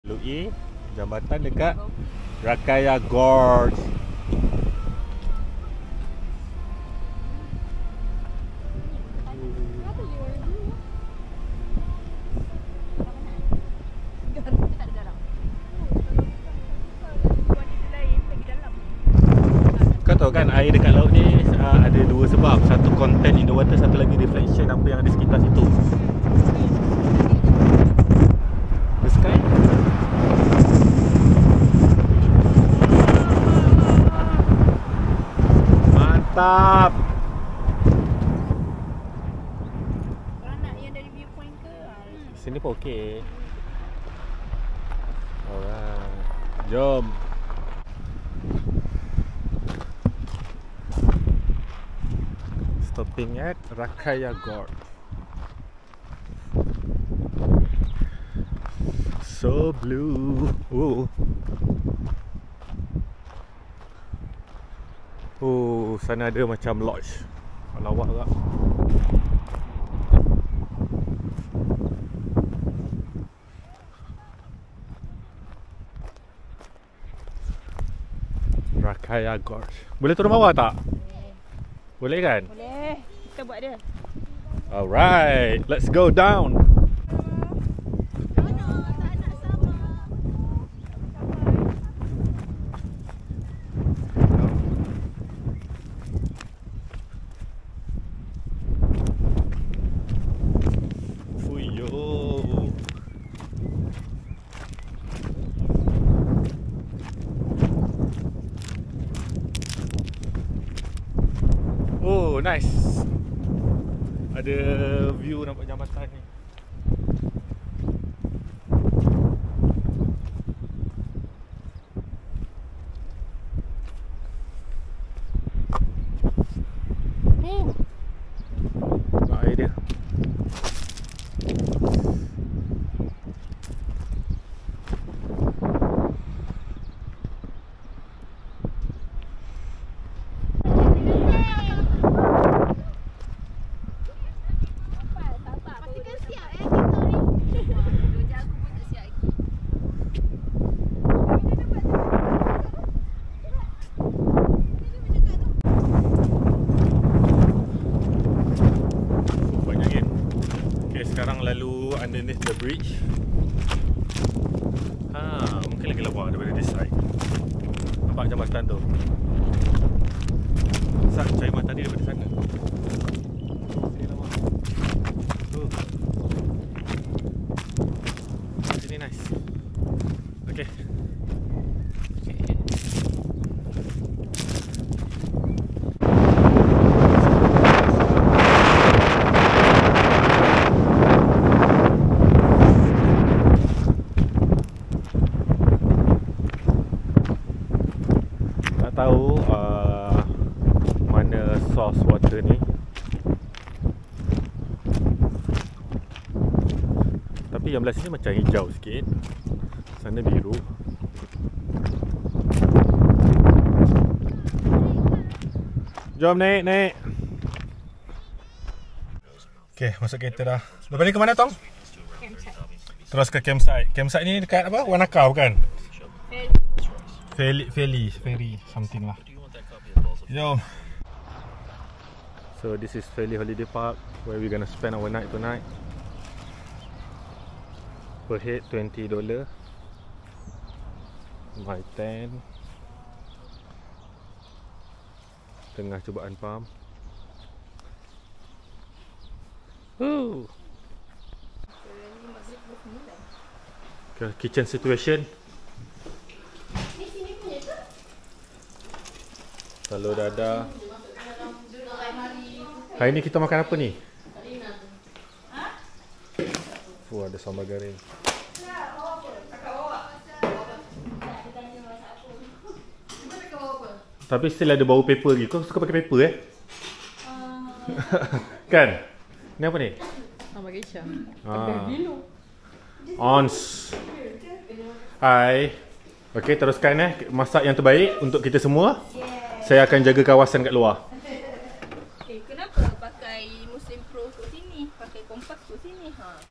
Day 1: Rakaia Gorge | South Island, NZ
from Castle Hill to Rakaia Gorge then straight to Fairlie where we camped for the night